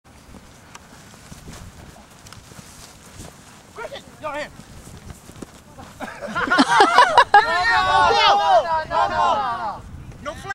Flag Football 26
Paul taking Chris down